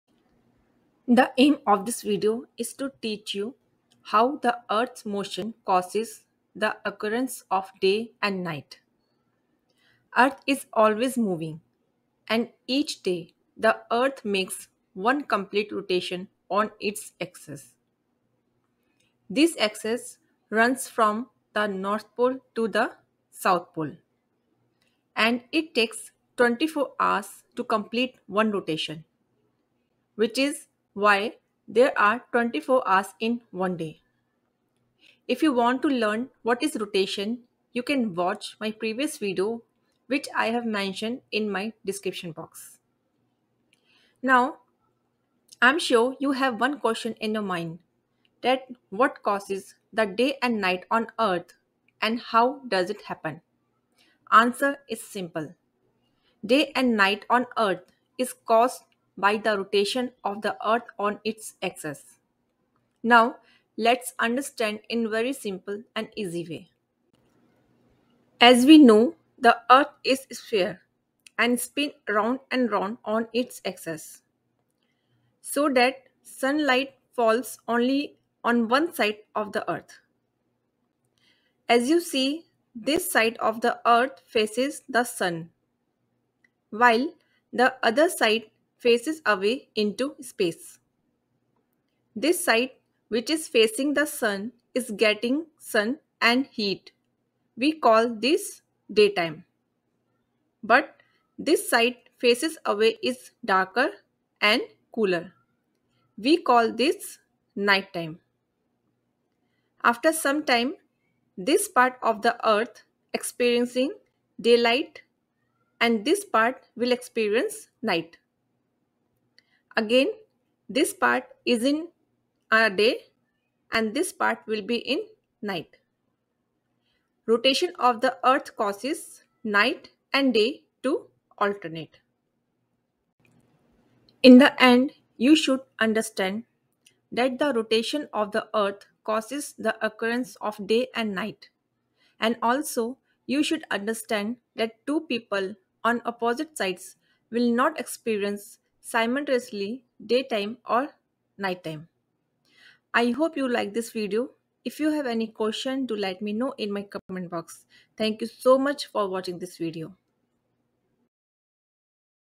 DAY & NIGHT ON EARTH | WHAT CAUSES DAY & NIGHT
As the Earth moves around the Sun it rotates on its axis, so we have day and night. The side of the Earth facing the Sun is bathed in light and heat (daytime). The side of the Earth facing away from the Sun, out towards space, is darker and colder (nighttime).